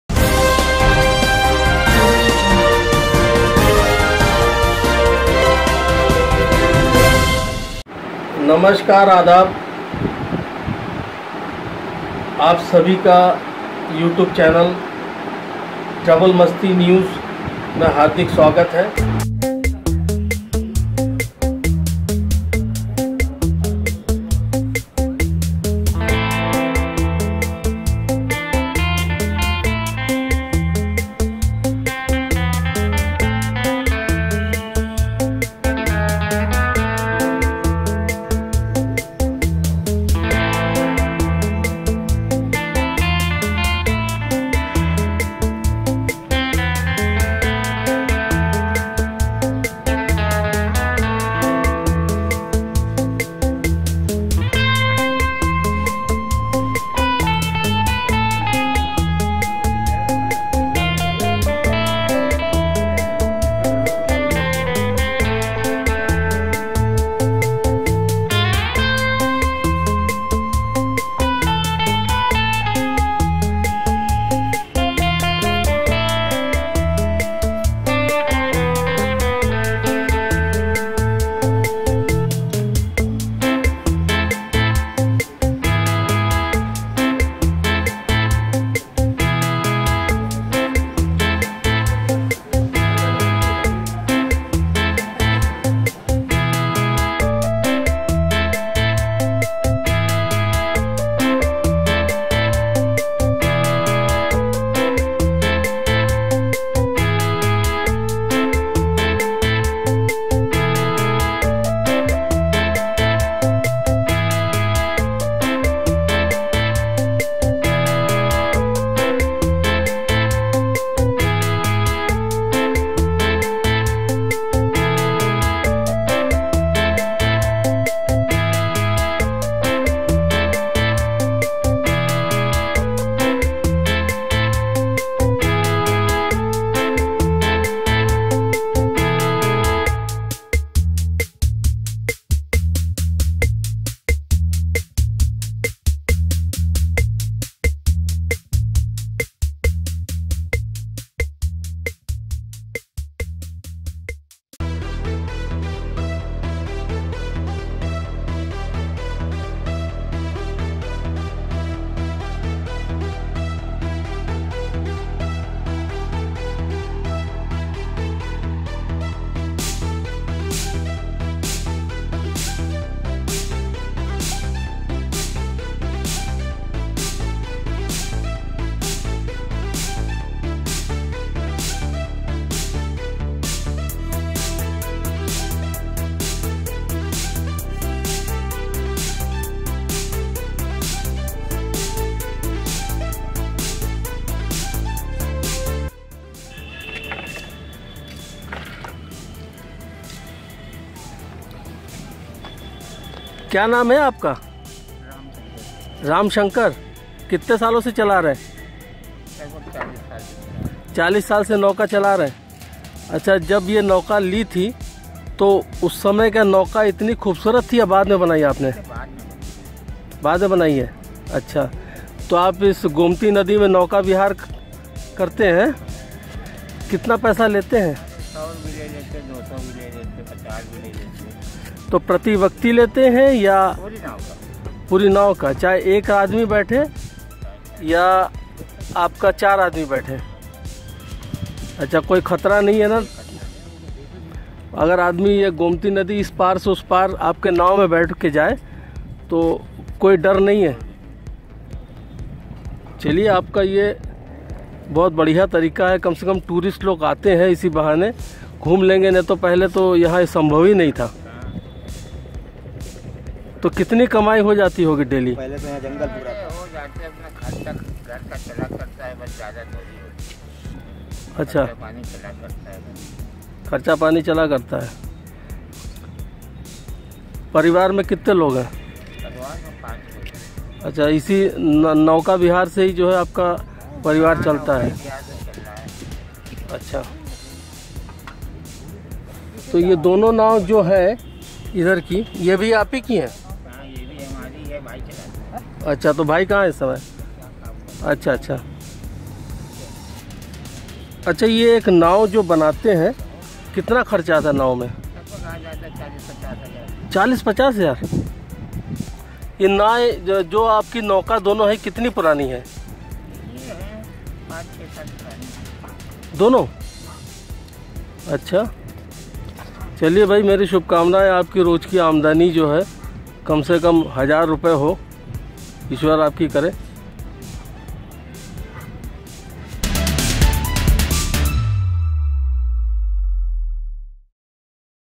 50 हजार का नौकाTRAVELMASTI
ये विडिओ लखनऊ से हमारे मित्र श्री रतीन्द्र नाथ चौधरी जी ने भेजा है यदि आपके पास भी unic विडिओ है तो मेरे व्हाट्सअप नंबर पर भेज सकते हैं मई उस विडिओ को अपने चैनल ट्रेवल मस्ती में प्रसारित कर दूंगा /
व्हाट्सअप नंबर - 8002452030